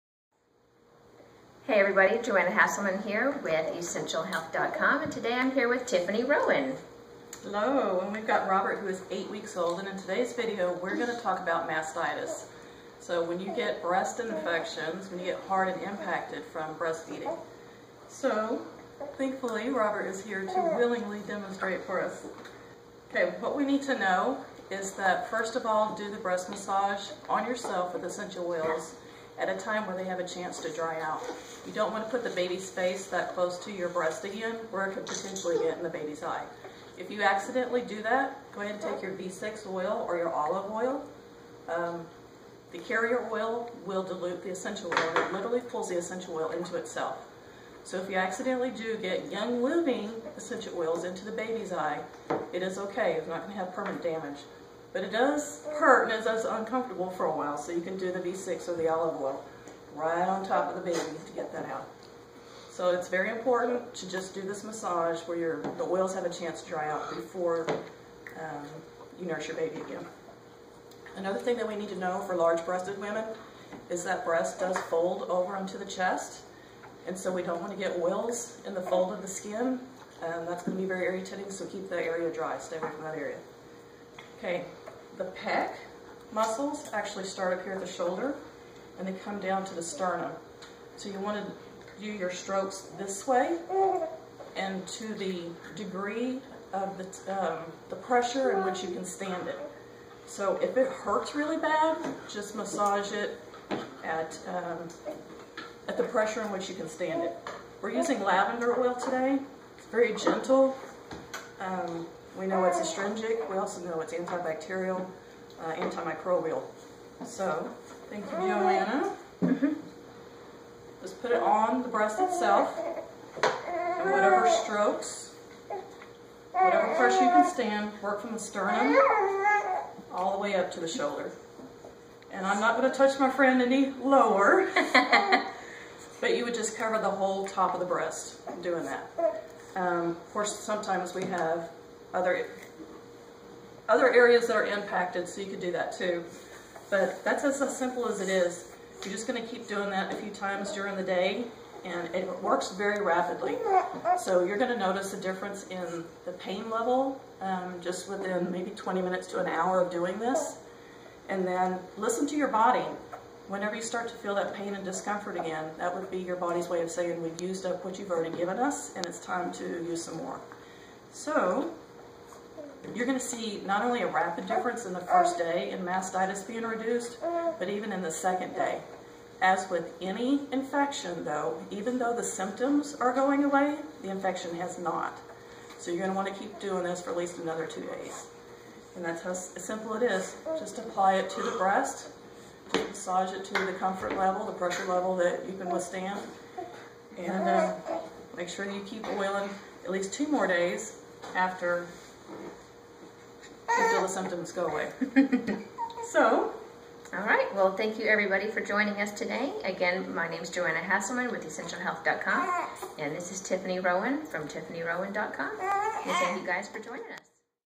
Essential oils for breast discomfort when breastfeeding
How to use Young Livings essential oilsand massage to ease breast discomfort when breastfeeding with special guest Tiffany Rowan